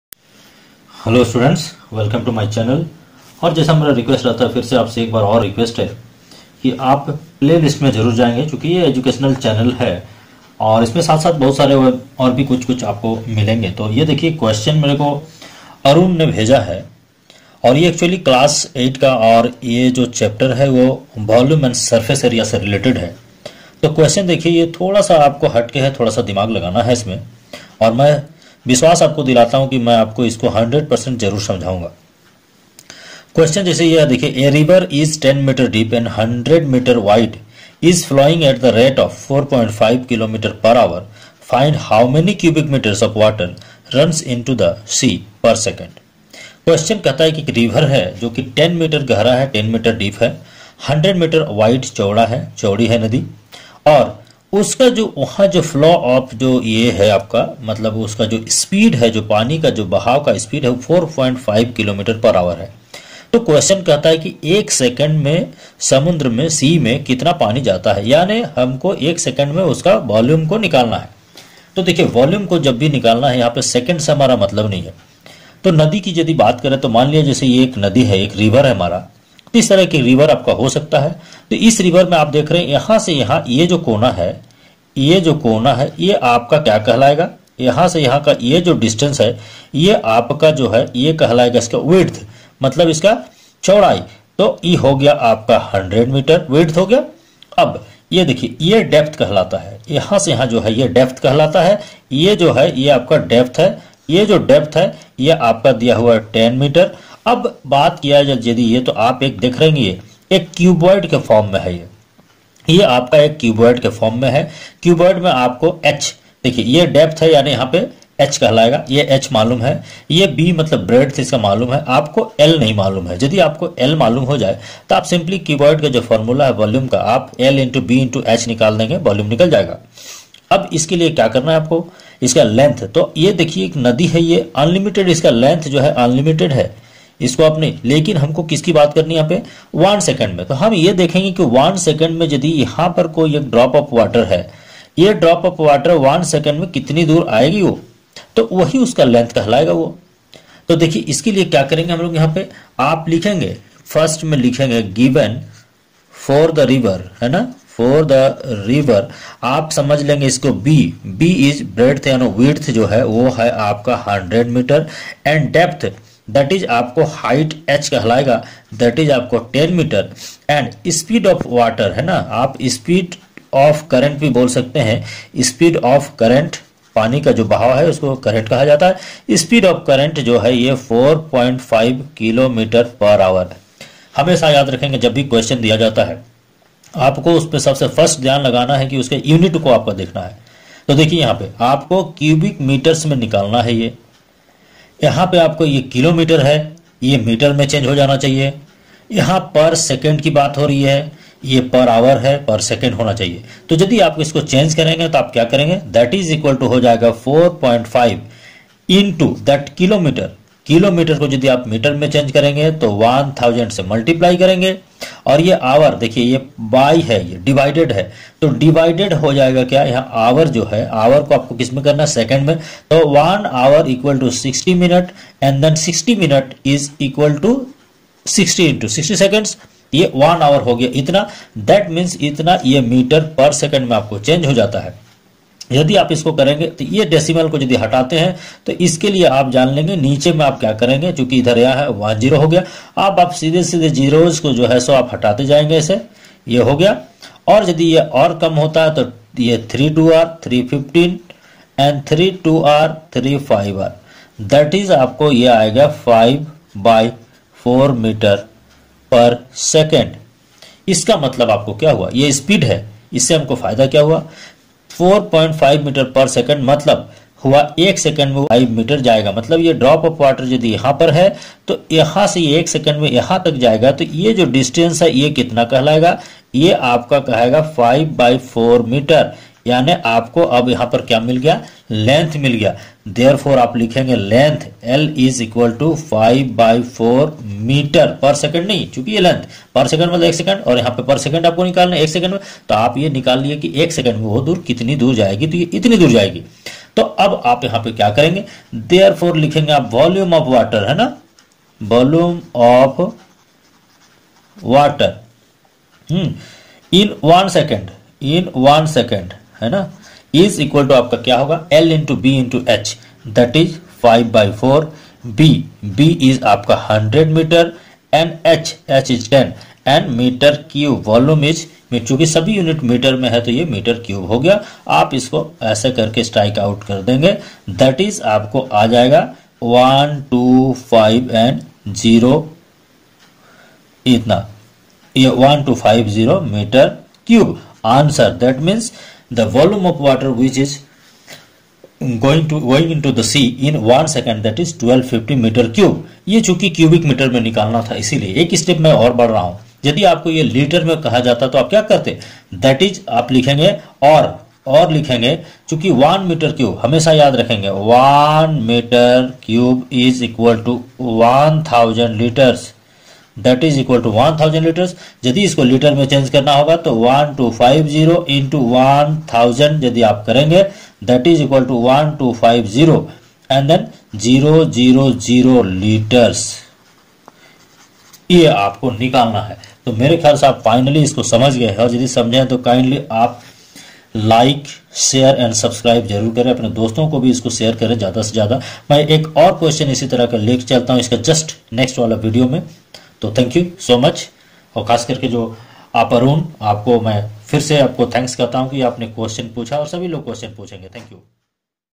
A river 10 m deep and 100 m wide is flowing at the rate of 4.5 km/h .Find how many cubic metres .
A river 10 m deep and 100 m wide is flowing at the rate of 4.5 km/h .Find how many cubic metres of water runs into the sea per second.
by rajeshN sir